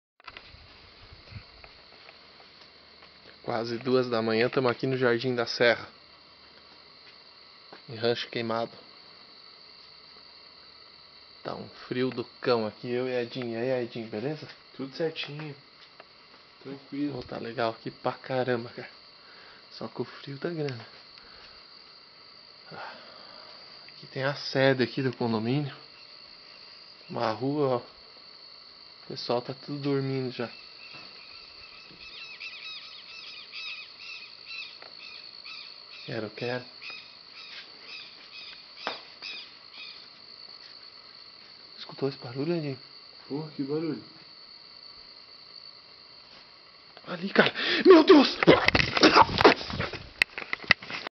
SERIAL KILLER, Rancho Queimado (Brazil)
Vídeo gravado em 2012, mostrando o ataque de um serial killer, em Rancho Queimado (Brasil).

"Video recorded in 2012, showing the attack of a serial killer in Rancho Queimado (Brazil)".